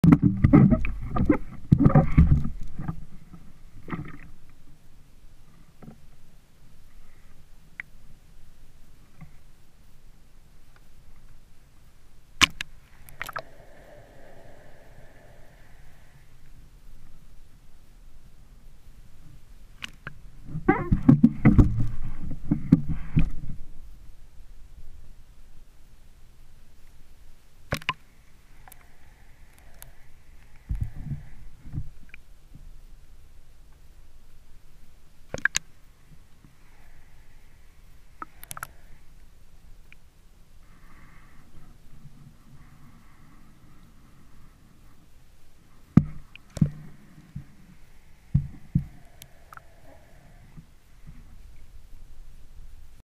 The Gull and the GoPro
I was surprised this gull tolerated the close proximity of the GoPro and the pole overhead.